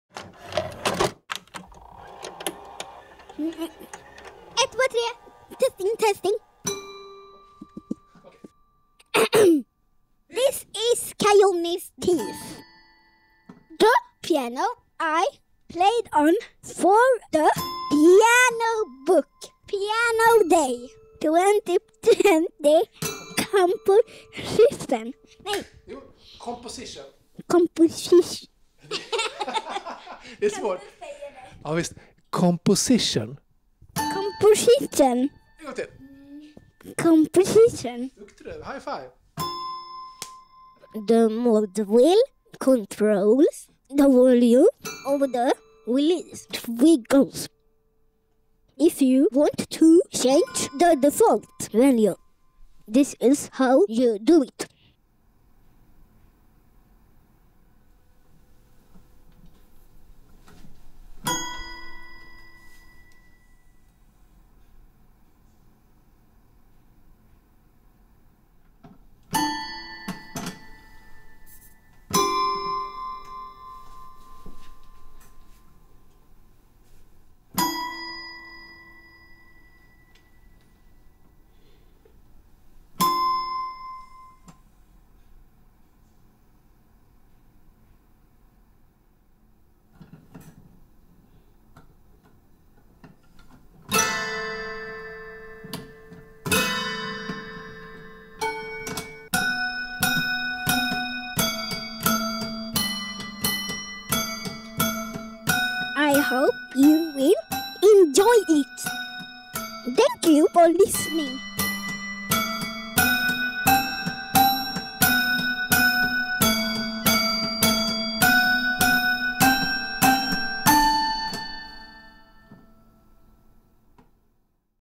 Khayones Keys Walkthrough
A walkthrough of Khayones Keys featuring Khayone as the narrator.

Khayones Keys, a tiny piano
3x round robin
2 velocity layer

Recorded with a Townsend Labs Sphere L22